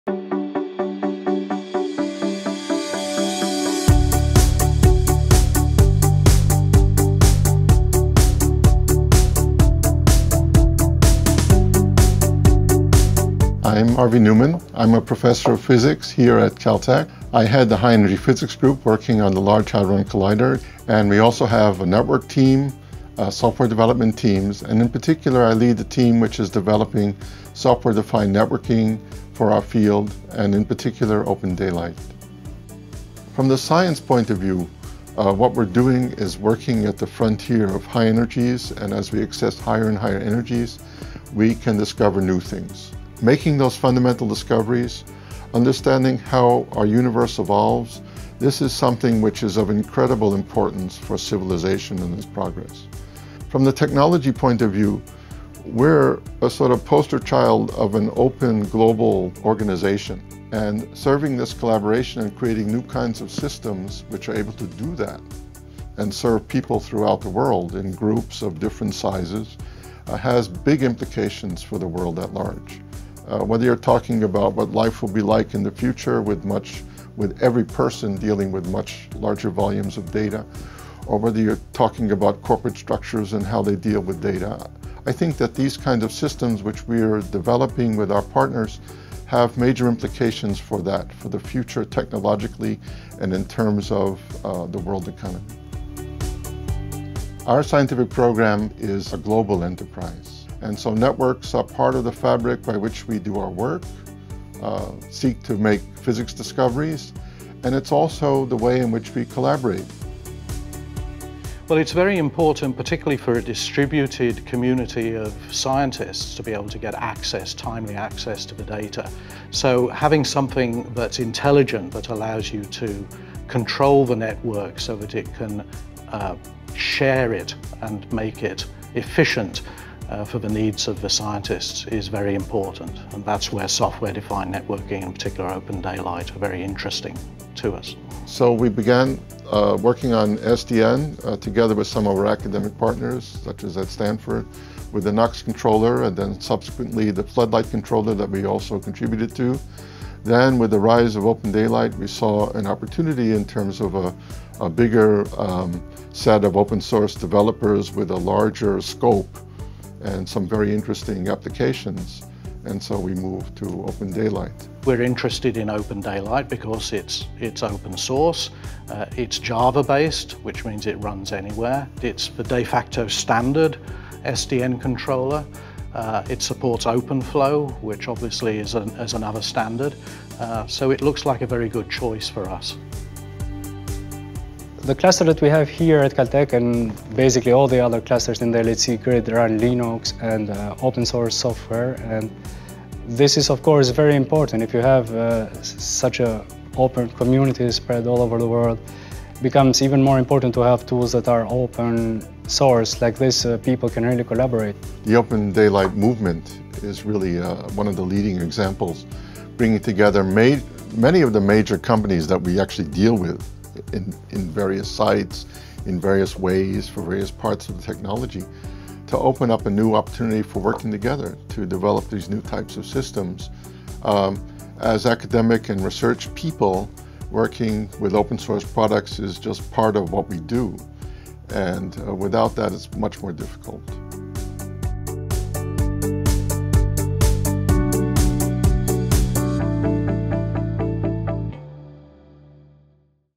Using Open SDN to Share Big Data from Large Hadron Collider
The world’s biggest machine-the Large Hadron Collider-is used to make new discoveries about how our universe evolves. It outputs petabytes of data shared with a global network of researchers in search of the Higgs Boson, extra dimensions, supersymmetry, and dark matter. Data sharing is being enabled by a highly intelligent Software-Defined Network (SDN) built with open source including OpenDaylight and Linux. We spoke with the high energy physics team at California Institute of Technology to understand the tech behind the LHC. Not only will their work have implications for science, but also for any enterprise, telco or service provider faced with ever-increasing data volumes.